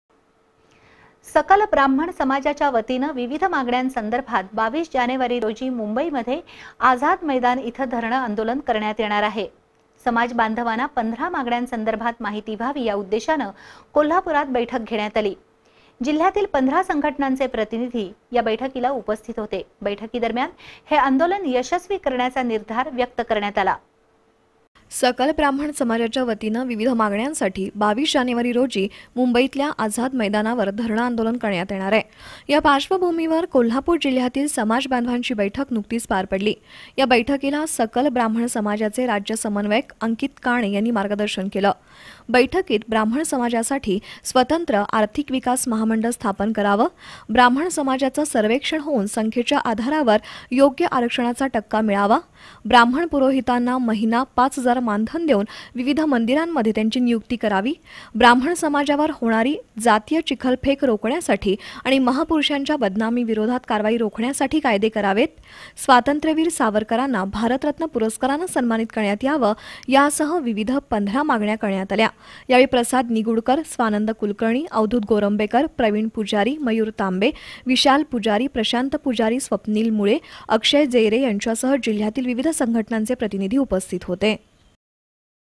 सकल ब्राह्मण समाजाच्यावतीनं विविध मागण्यांसंदर्भात २२ जानेवारी रोजी धरणं आंदोलन
सकल ब्राह्मण समाजाच्यावतीनं  विविध मागण्यांसंदर्भात २२ जानेवारी रोजी मुंबईत आझाद मैदान इथं धरणं आंदोलन करण्यात येणार
Channel B